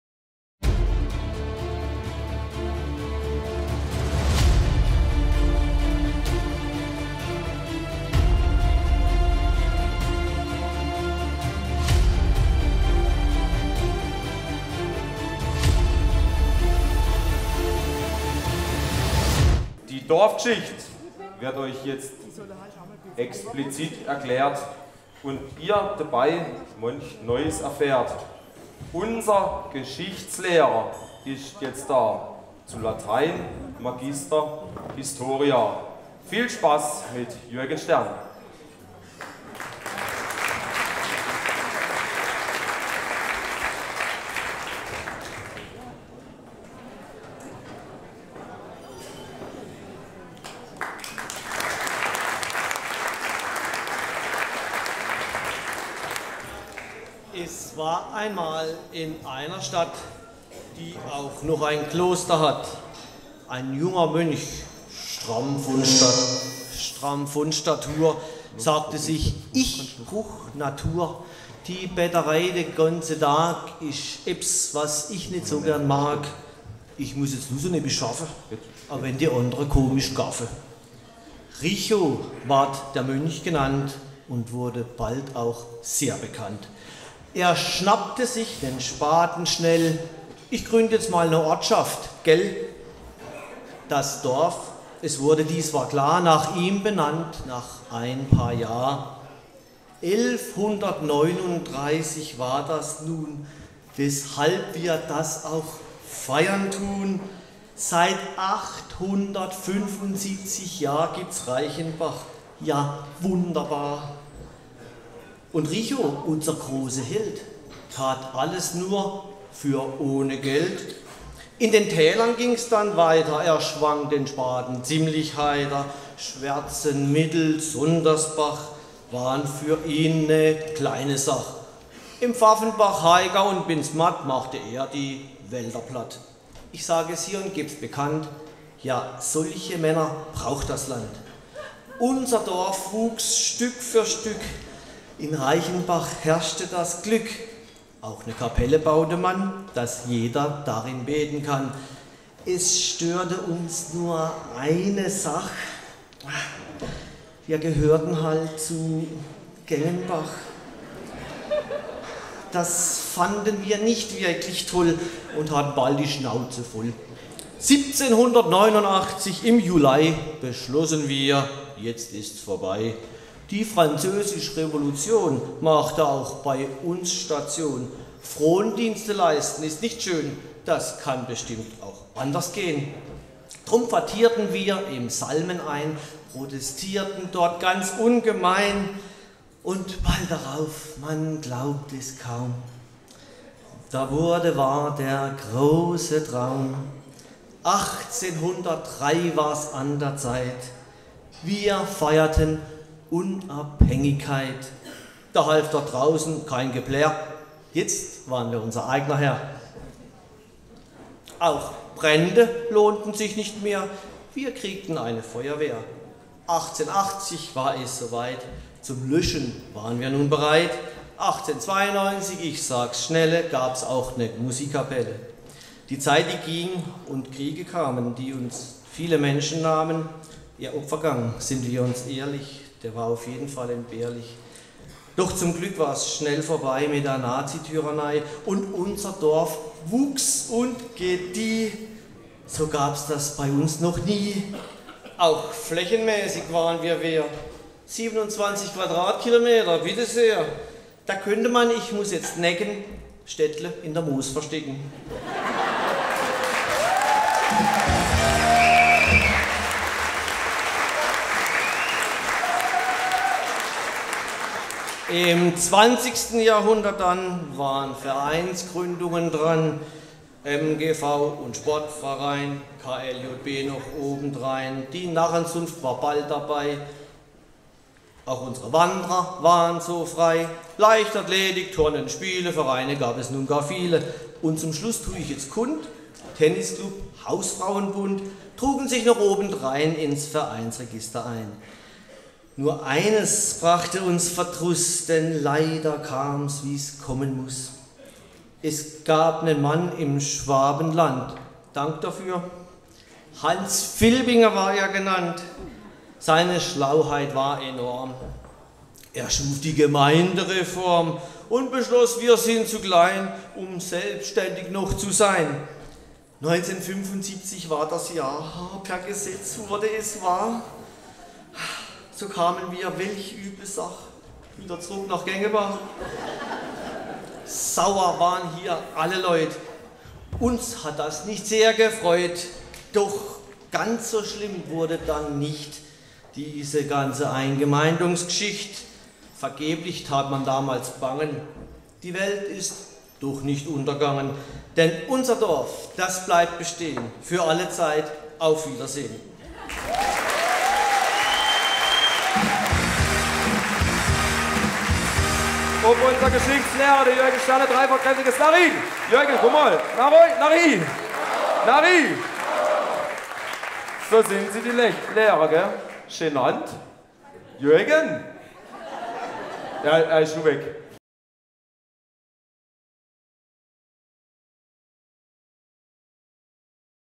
Geschichtslehrer (2014)
Unser Geschichtslehrer erzählt euch heute die Geschichte von Reichenbach.

Vorgetragen von Jürgen Stern

Teufelsbild im Intro und Outro: picslix-fotografie.de